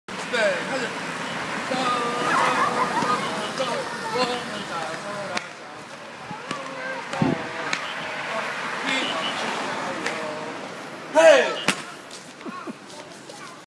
同手同腳的光頭佬
別看大白一副狠樣，跳起舞來卻是另一個鳥樣！